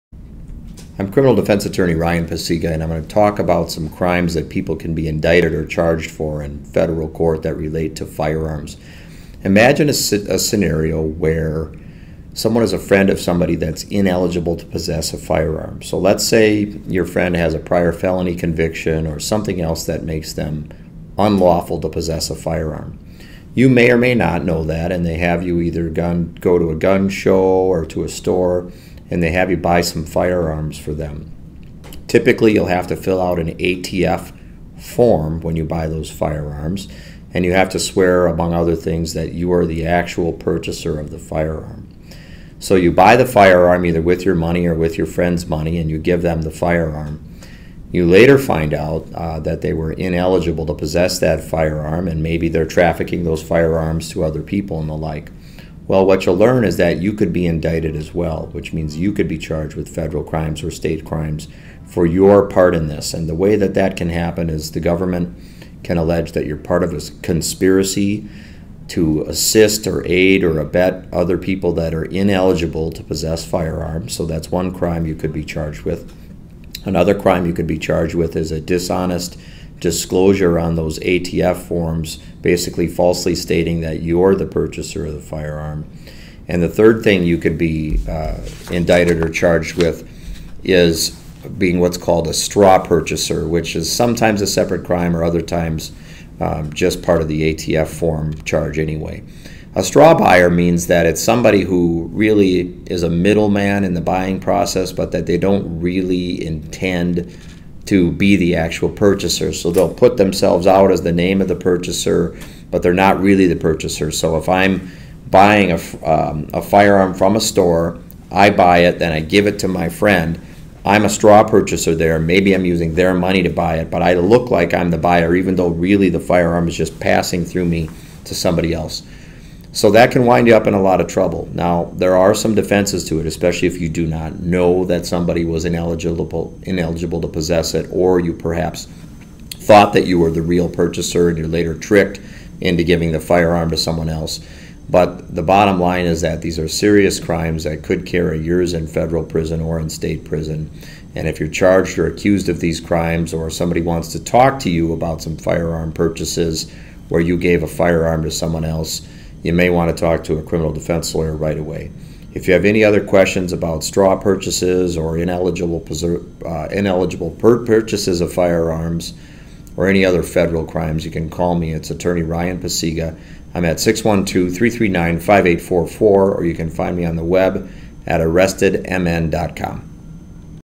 Federal Firearms Crimes
Federal firearms crimes are serious. Charges can include Conspiracy, Felon in Possession of Firearms under Title 18 United States Code Section 922(g)(1), and 18 United States Code Section 922(a)(6) for making a false statement during the purchase of a firearm. Often cases may involve a "straw buyer" who sometimes can be tricked into buying a gun for someone that is ineligible to possess a gun. A separate crime can be charged for a false statement on ATF form 4473. Federal criminal defense lawyer Ryan Pacyga explains these crimes and represents people accused of such crimes.